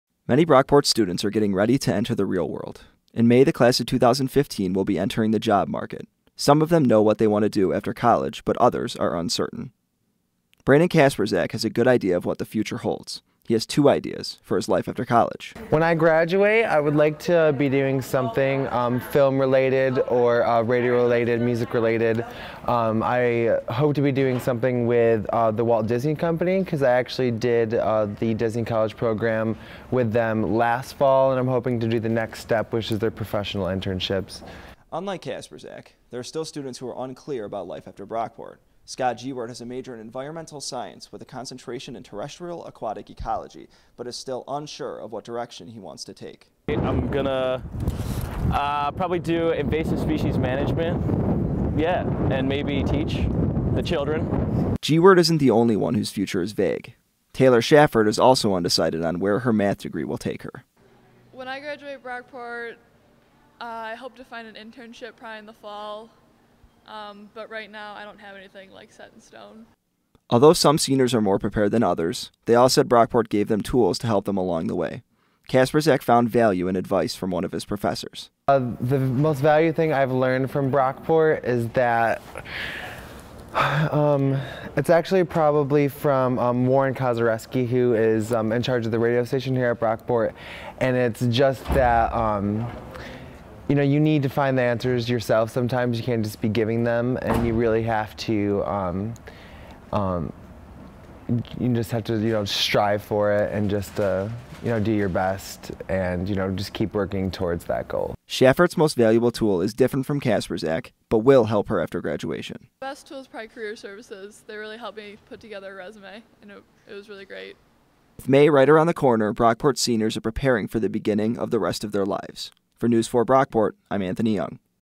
Anthony Young - Senior Experience TV Package
Television package about the senior experience at the College at Brockport.